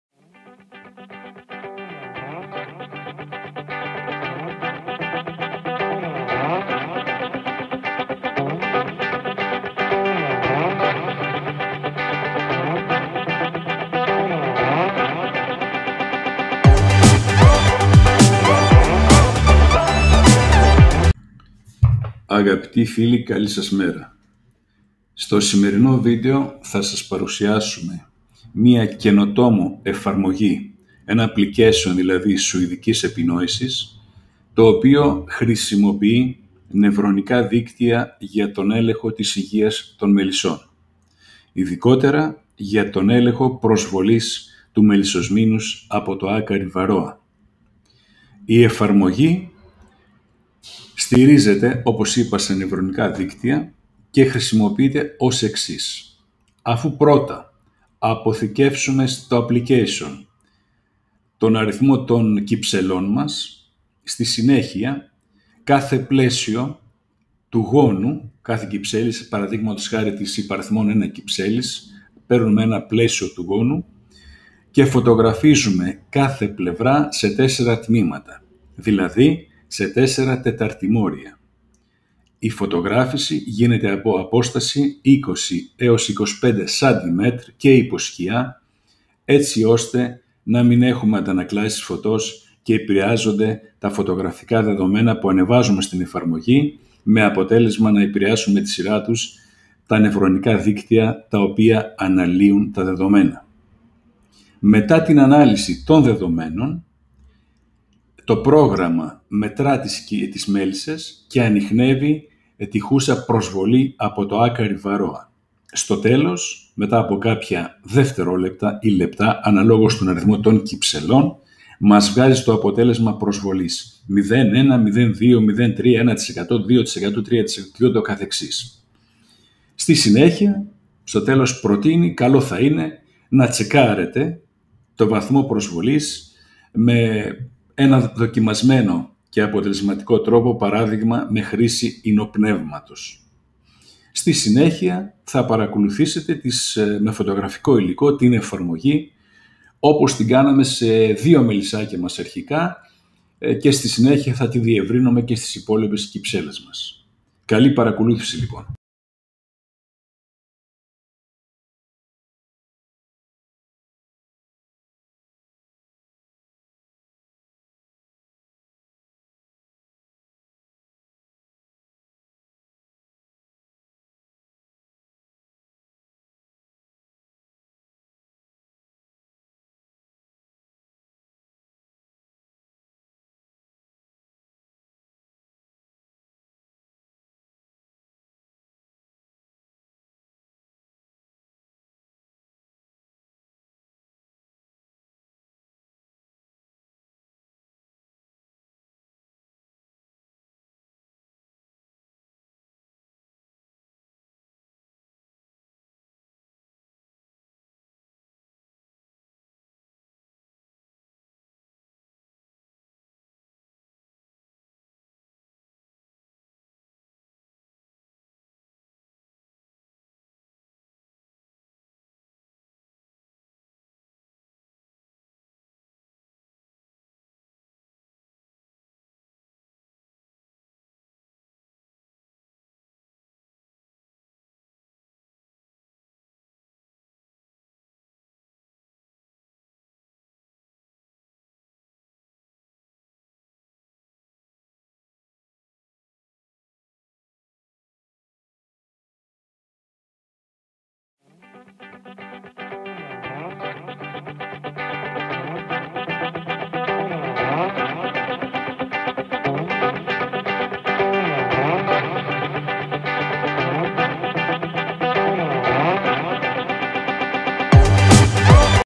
Καινοτόμος εφαρμογή(app) ελέγχου της υγείας των μελισσών
Παρουσιάζουμε μια καινοτόμο εφαρμογή σουηδικής επινόησης που χρησιμοποιείται για τον έλεγχο της υγείας των μελισσών.